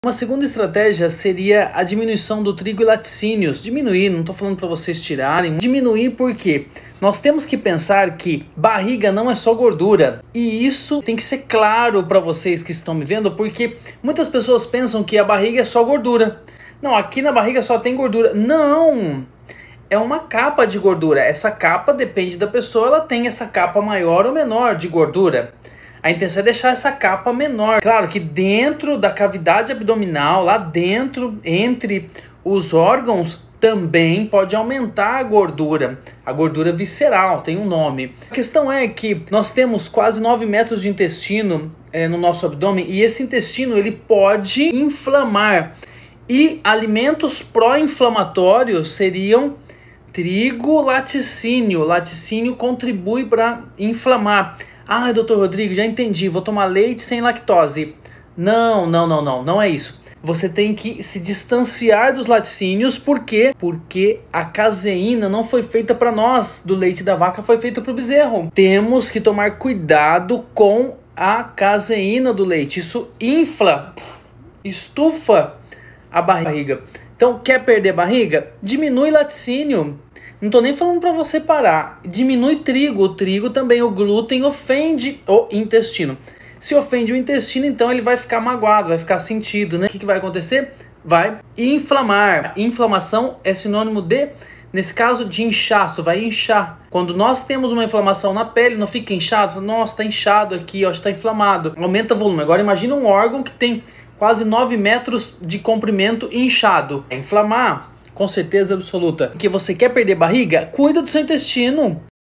COMO DIMINUIR A BARRIGA INCHADA
Entenda quais são os fatores que causam o aumento da barriga e como reduzir.